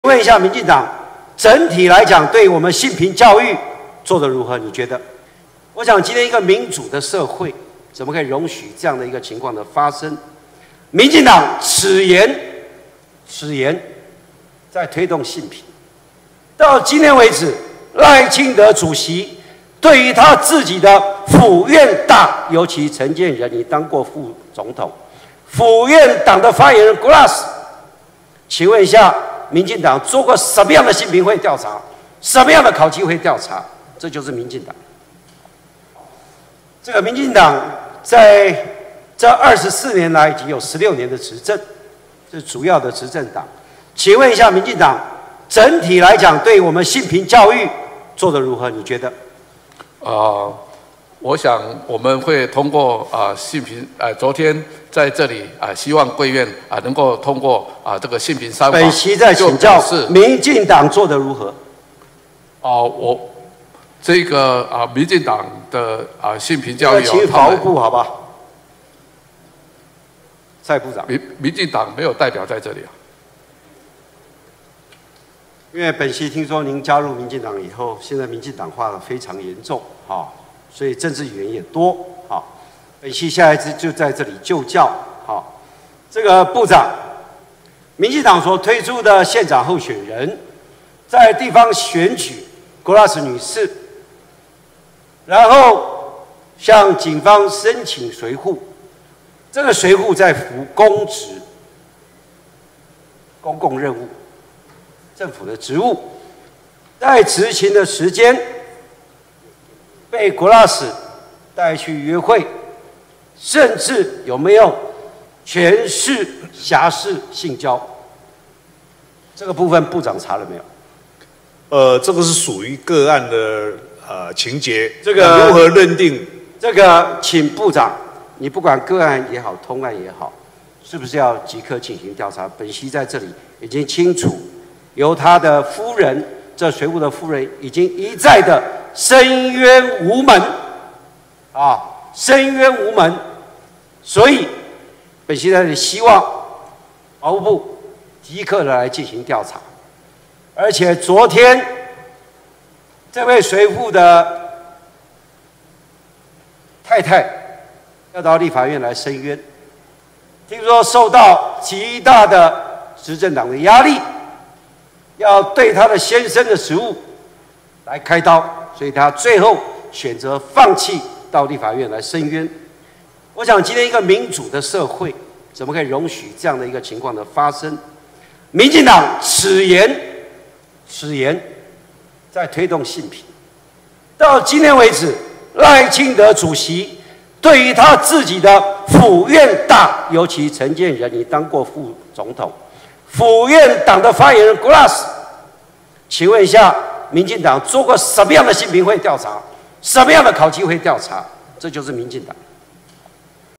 【扯！花蓮王 傅崐萁 性騷擾強吻女記者，竟在國會砲轟 執政黨性平教育失敗！ 】｜國會調查兵團 新聞台 cic.tw
日前國民黨立委傅崐萁身陷性平爭議，遭媒體人指控涉嫌對她性騷擾，不過今(19)日傅崐萁，不只問行政院長陳建仁「民進黨對性平教育做得如何」，陳建仁直接回應「現場沒有民進黨」，帶隨扈去約會，這讓坐在台下的陸委會主委邱太三忍不住笑了出來。

【兵團告急 需要您的贊助支持 ！懇請支持監督 揭露國會 守護台灣民主～ 】五種支持我們的方法